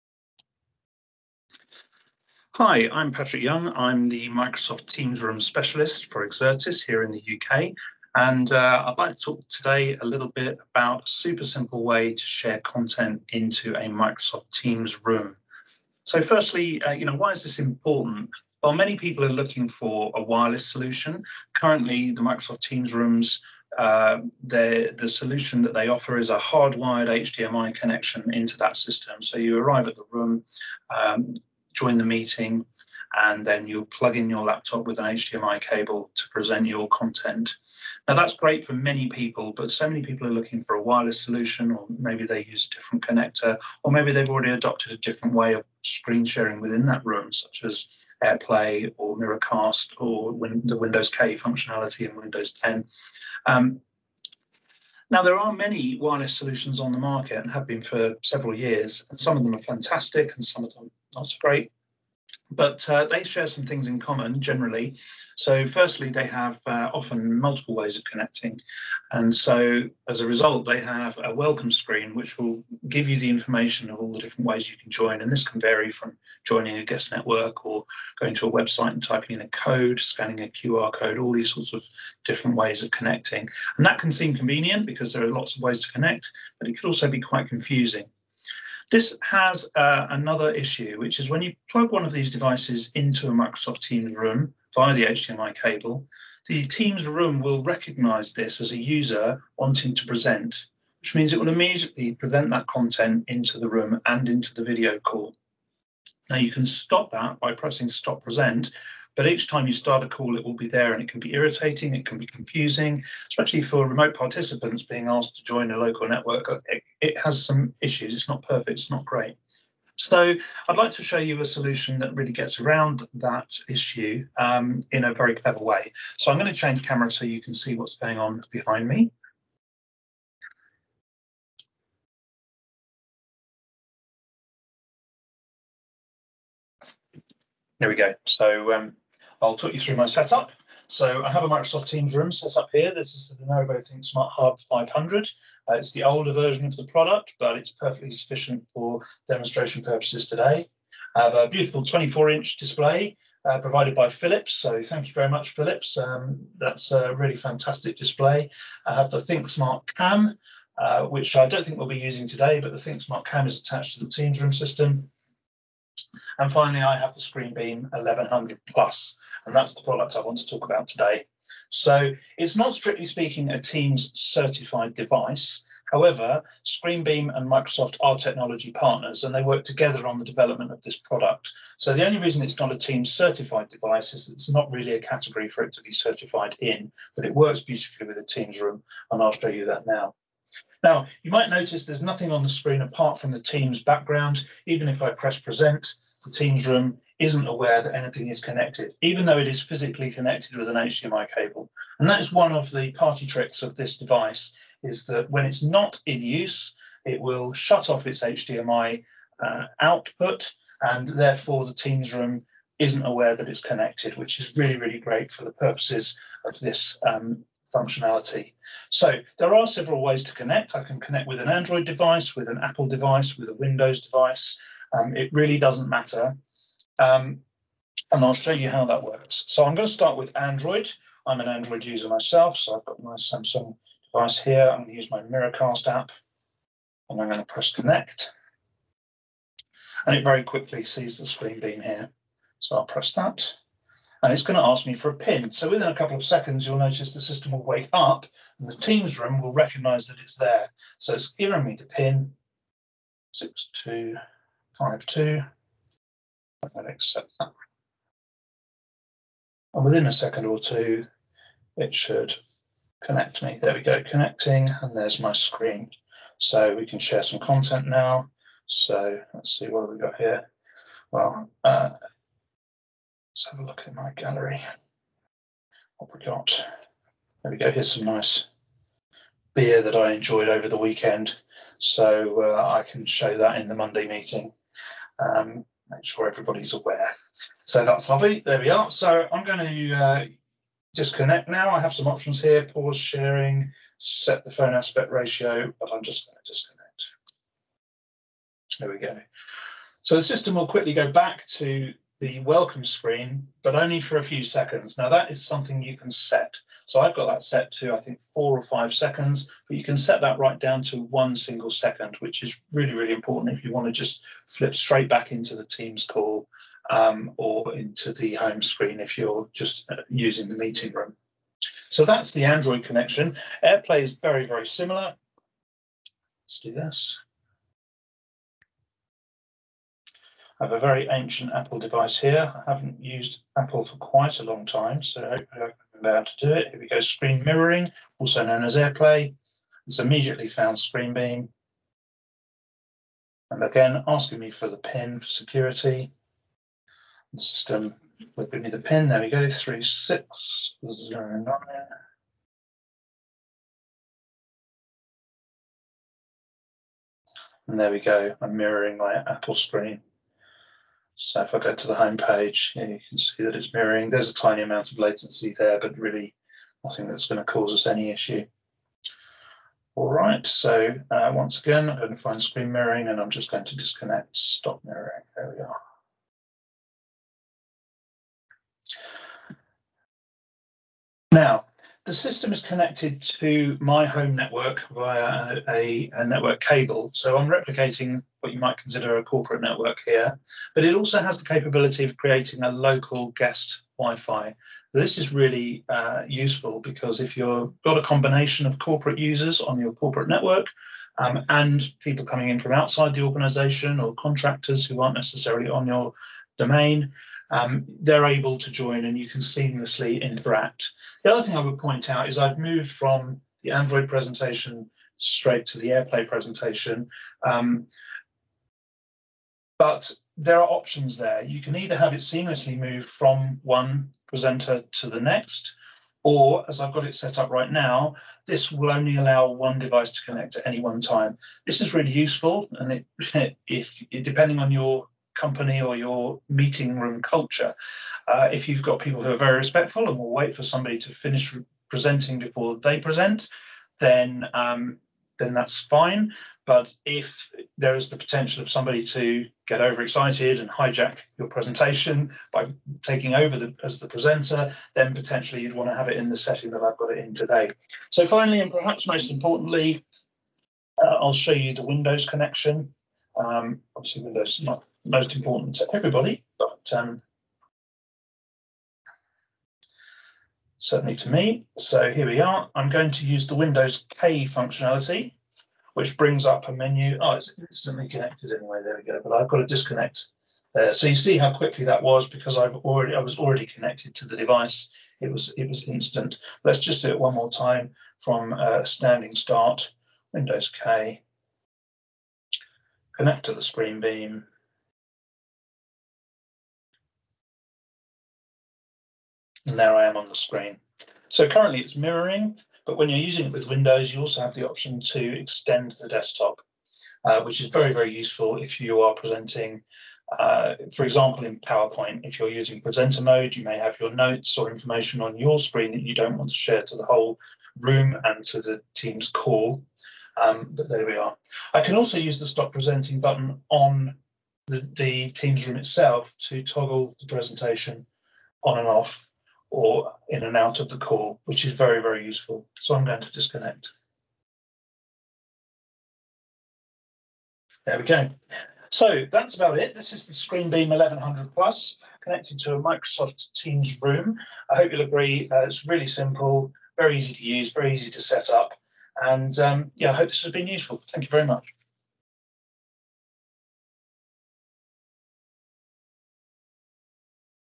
Teams Rooms with ScreenBeam
A super simple way to add wireless presenting to your Microsoft Teams Room